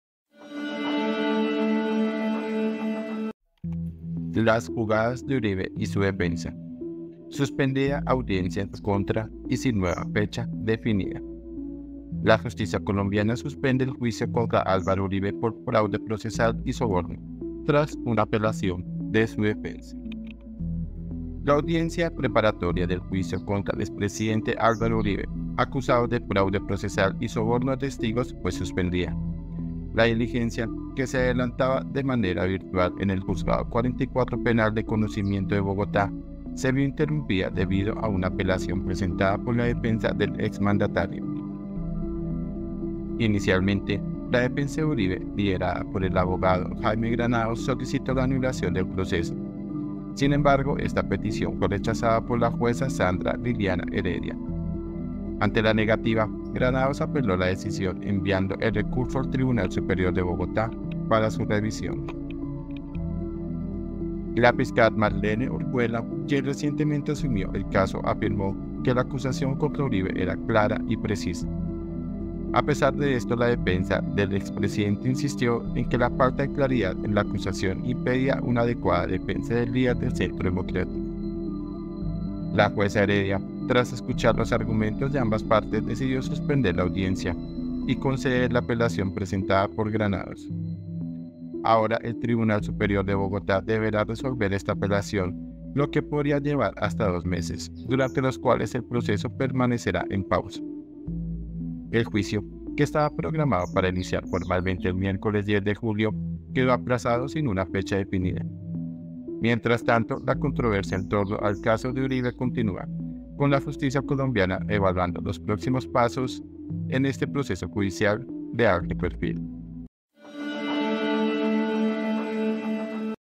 LAS JUGADAS DE URIBE Y SU DEFENSA 🔴 Suspendida audiencia en su contra y sin nueva fecha definida
La justicia colombiana suspende el juicio contra Álvaro Uribe por fraude procesal y soborno, tras una apelación de su defensa.
AL PUNTO